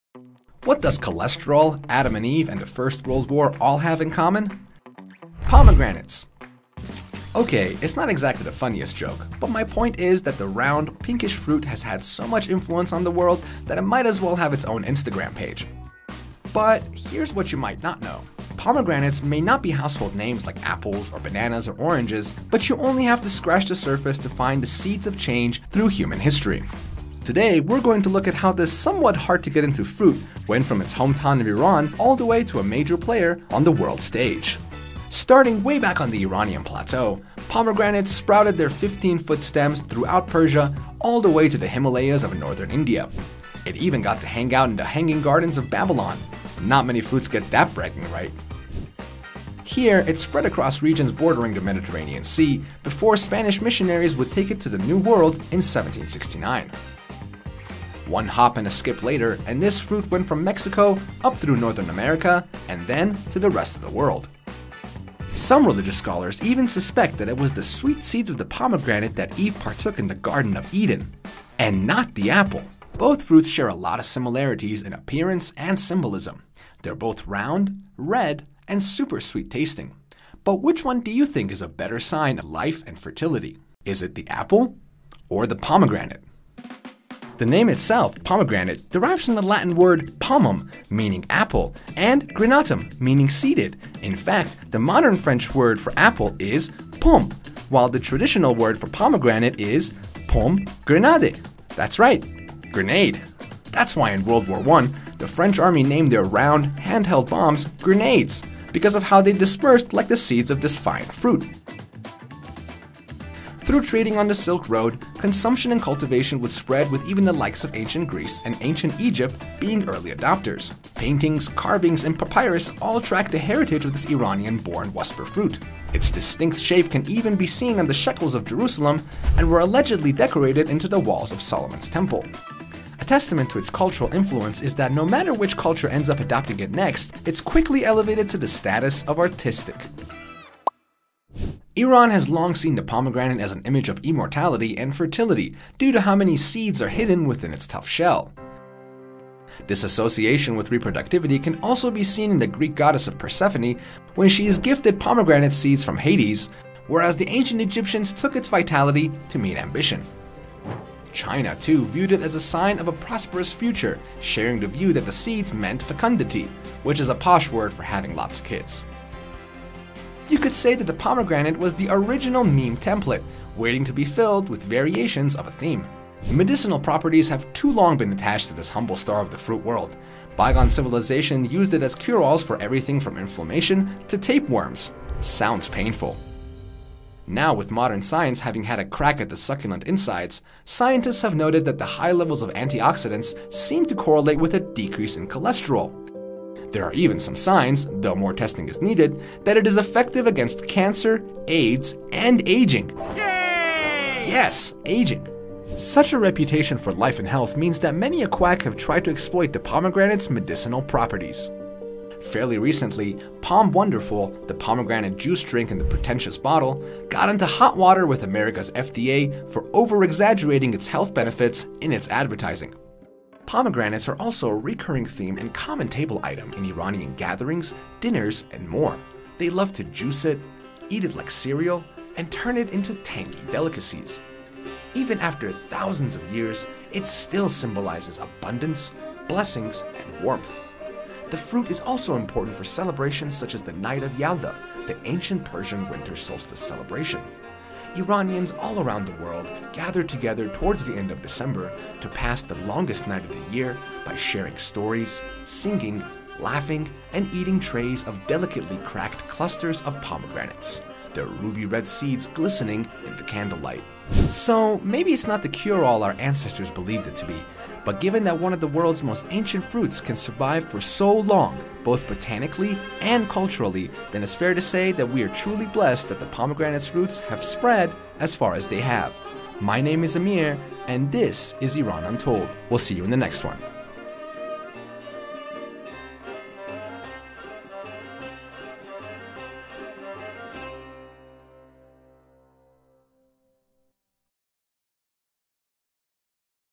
The Secrets of Pomegranates
The jewel of winter ❄️ , pomegranate, is the national fruit of Iran 🇮🇷. It is a symbol of love ❤️ , beauty 🥰 and fertility 🤰🏻. It is even a part of the Iranian mythology which tells that Esfandiyar became an invincible hero 🦸🏻‍♂️ after he had a pomegranate. For Zoroastrians (the followers of the oldest remaining Iranian religion), the pomegranate symbolises the soul’s immortality 😇and the perfection of nature 🍃.